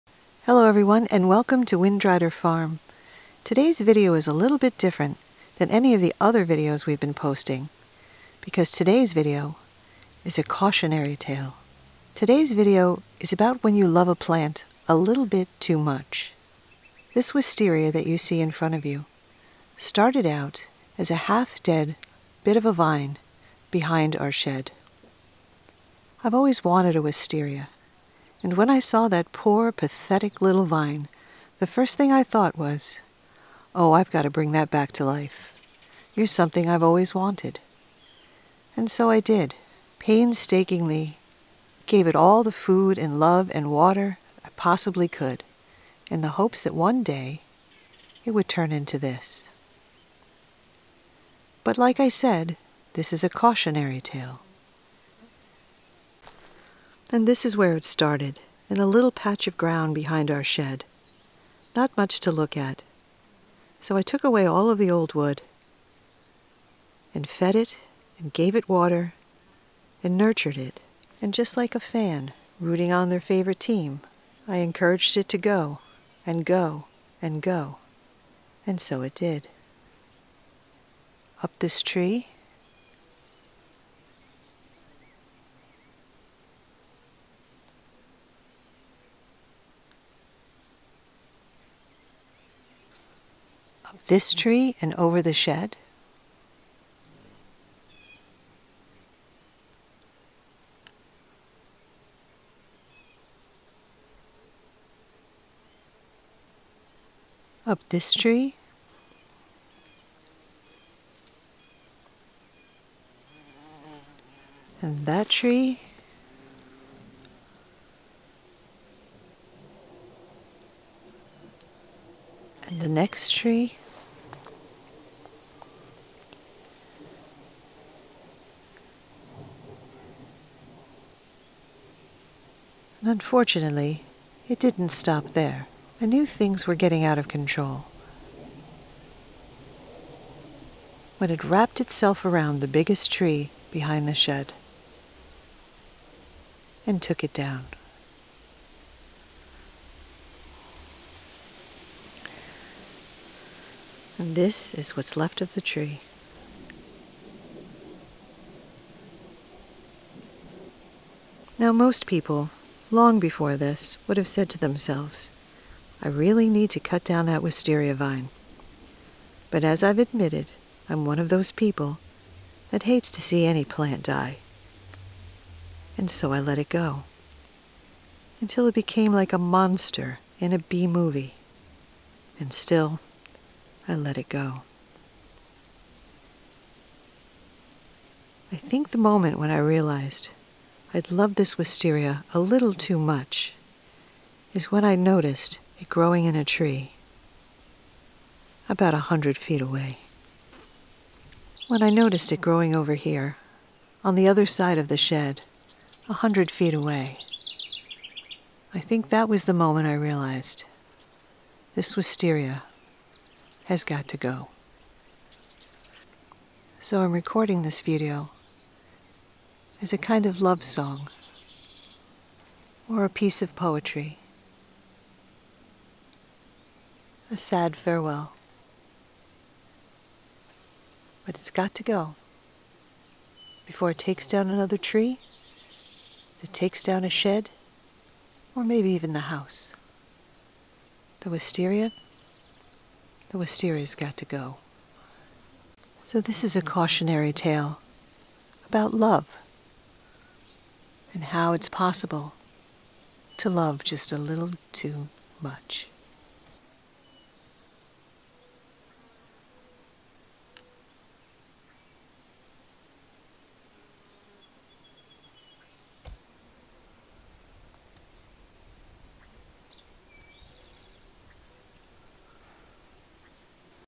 Wisteria - A cautionary tale
When your wisteria vine turns into a monster from a B movie.  When we bought our farm, we noticed a pathetic wisteria behind our shed and nurtured it back to life.  This video shows how out of control a wisteria can become if you are intoxicated by it's beauty. A cautionary tale.